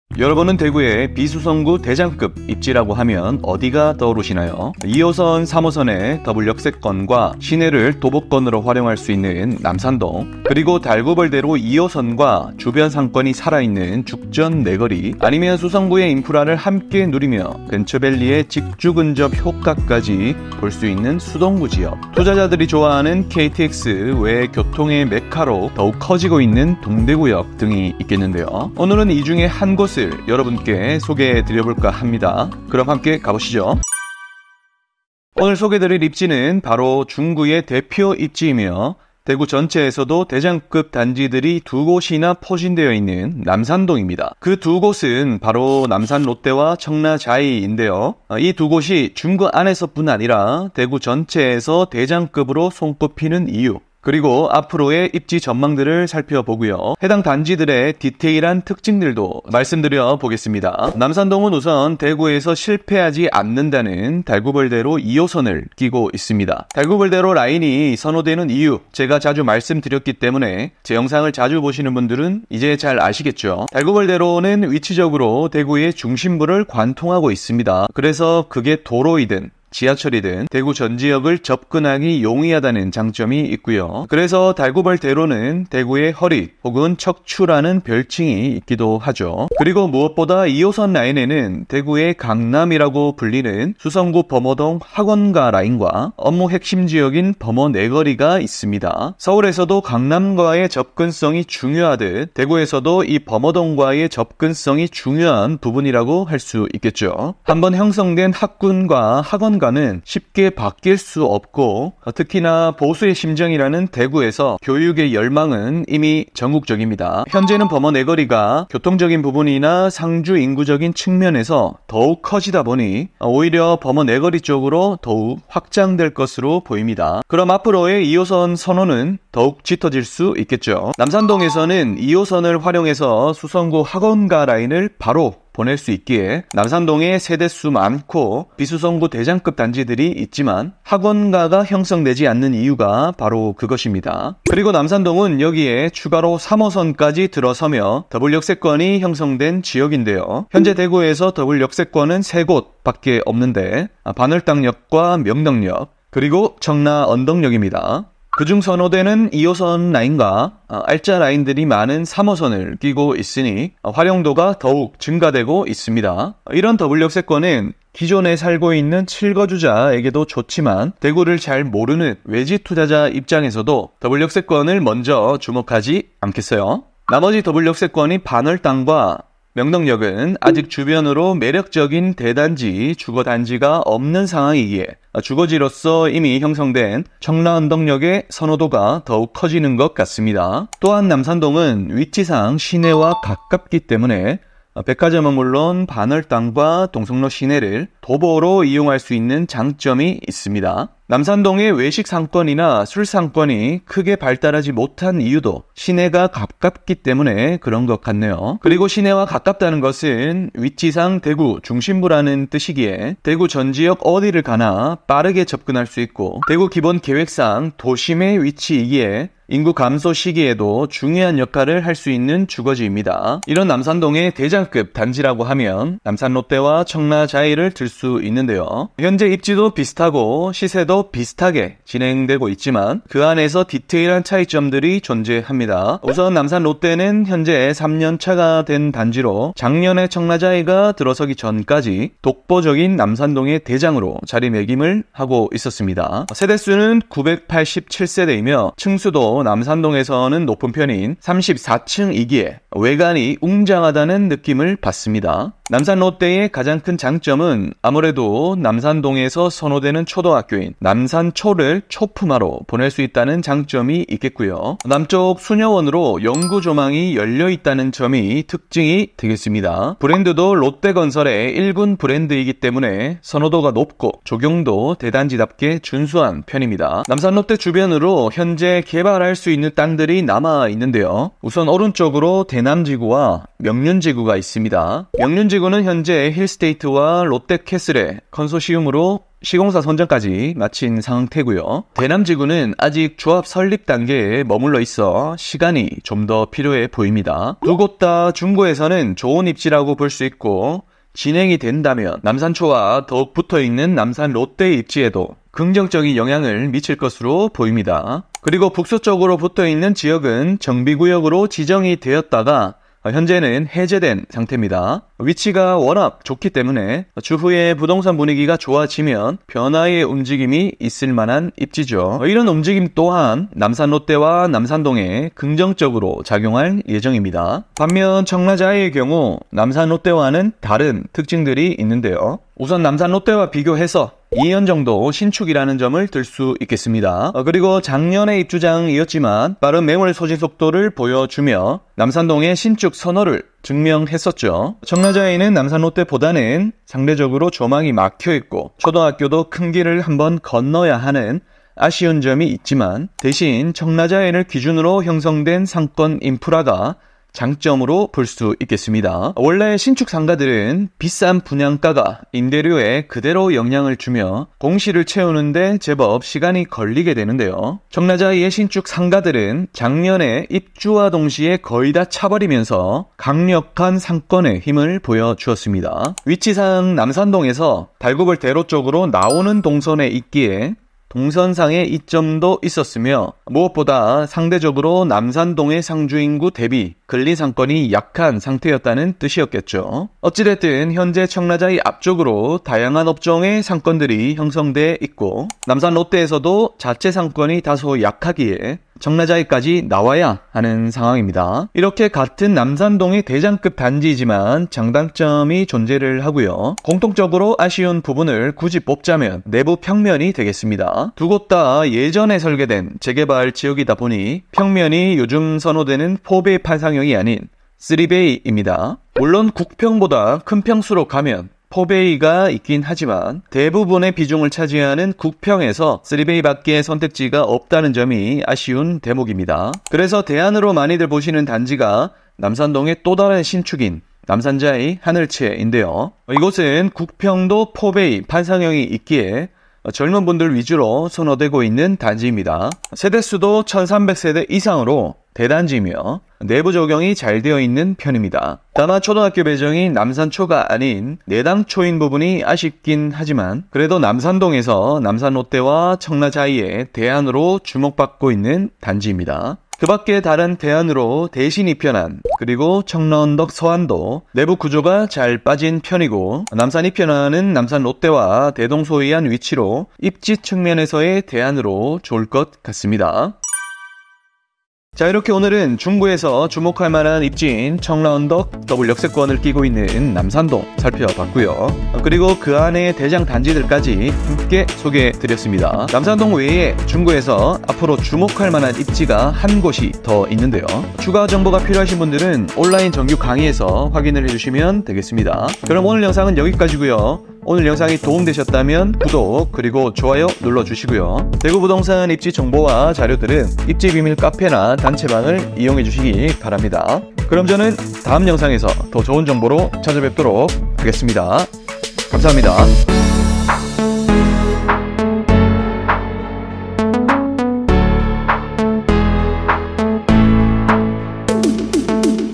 비수성구 대장이라고 불리는 남산동 도대체 왜 인기가 있을까?ㅣ대구부동산 주목할입지 중구편
대구에서 비수성구 대장급 입지라고 하면 떠오르는 지역들이 있습니다.
그중 오늘 소개드릴 지역은 바로 남산동인데요.

실거주 뿐만 아니라 외지투자자분들에게도 선호되는 청라언덕역 남산동!
오늘 그 이유와 남산동의 전망, 그리고 대장단지들의 디테일한 특장점들도 정리해드리겠습니다.
도움이 되셨으면 좋겠습니다.

00:00 영상시작
01:00 남산동 입지분석
03:43 남산롯데 입지
05:27 청라자이 입지
06:47 아쉬운 점 및 대안

* 본 영상에서 주로 활용된 지도는 맛동산개발지도입니다.
* 대발이부동산TV로부터 썸네일 자료를 제공받았습니다.

(비번: 7915)